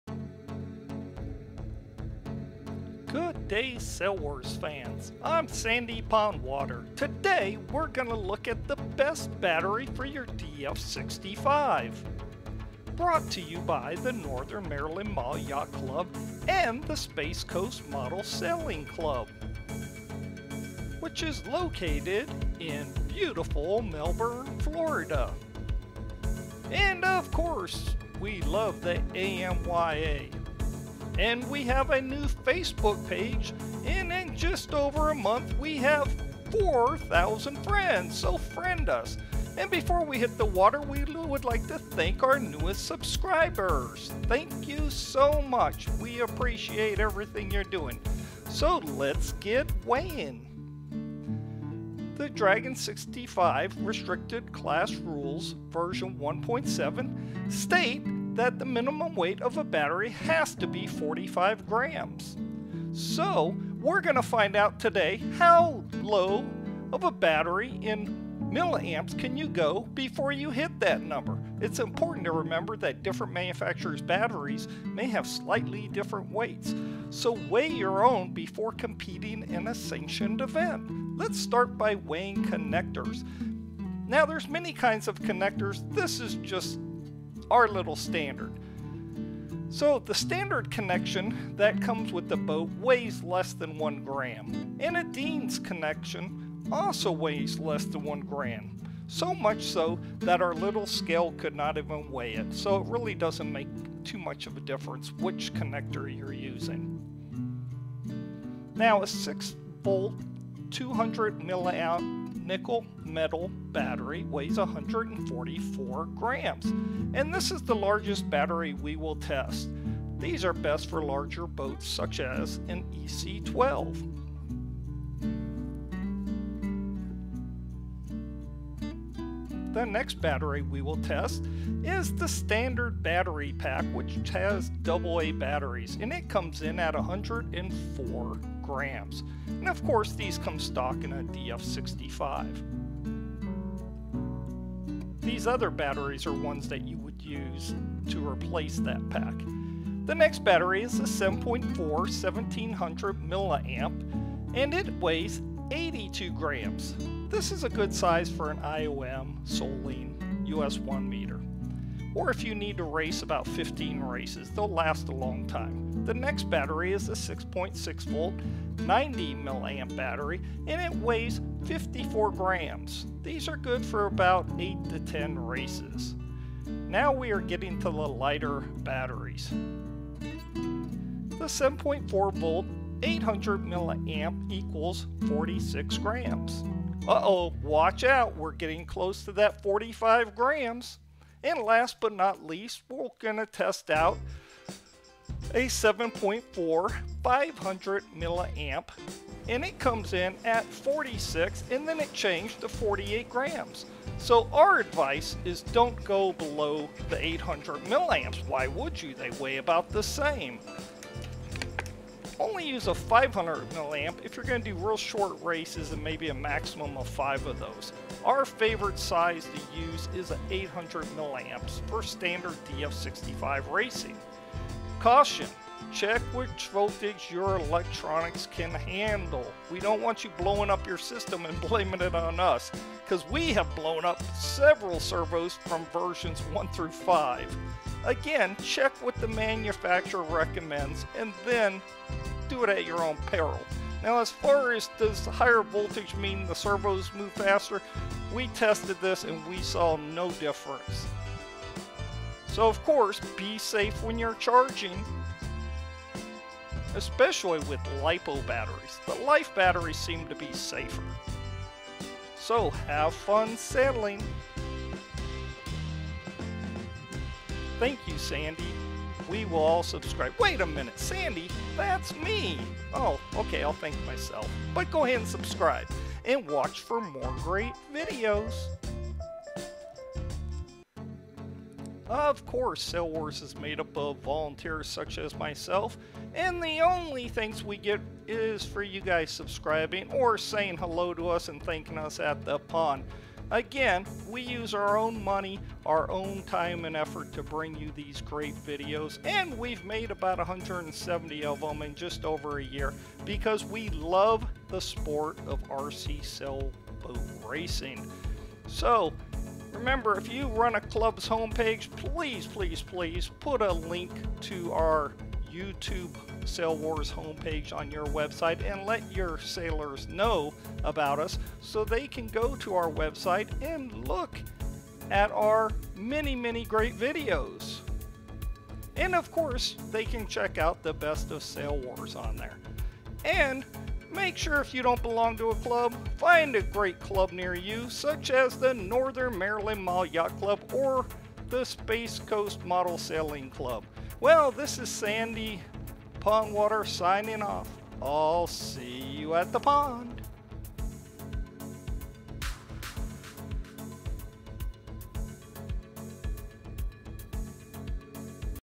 Sail Wars Science! Best Battery for your DF65, Video#169
Sail Wars tests out different sizes of batteries for your DF65.
Sail Wars features races for the following classes:  Soling, DF65, DF95, US1M, ODOM, IOM, SeaWind, EC12, as well as many other RC Sailboats.  We have playlists for many different kinds of RC Yachts.